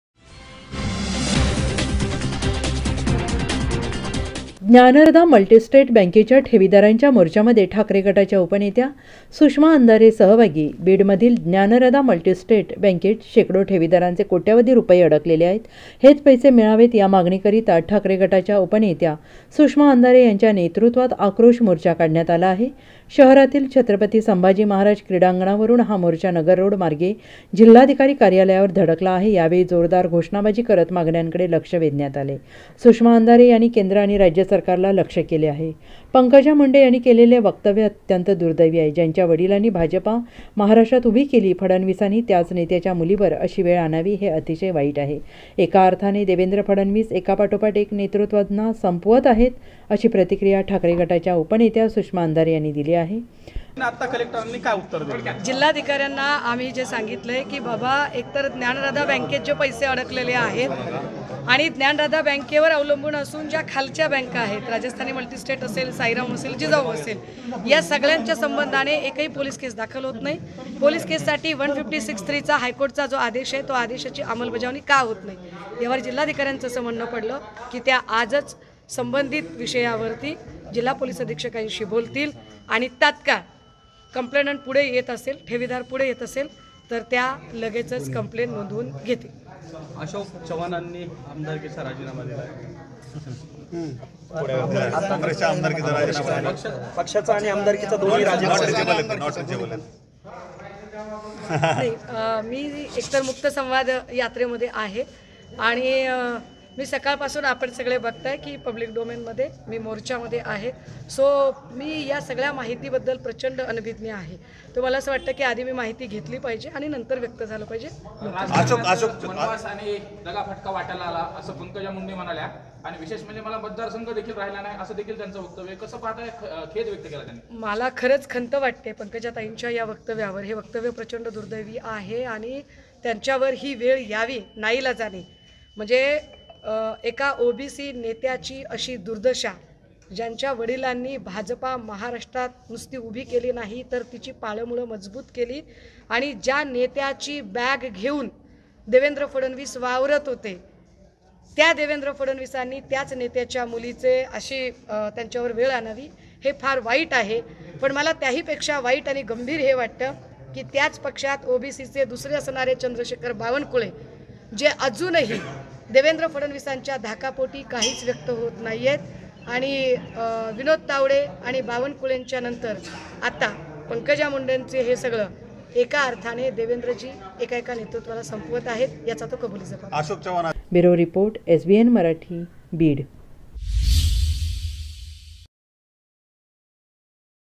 SUSHMA ANDHARE | बीडमधील ज्ञानराधा मल्टीस्टेट बँकेच्या ठेवीदारांच्या मोर्चामध्ये सुषमा अंधारे सहभागी
बीडमधील ज्ञानराधा मल्टीस्टेट बँकेच्या ठेवीदारांच्या मोर्चामध्ये ठाकरे गटाच्या उपनेत्या सुषमा अंधारे सहभागी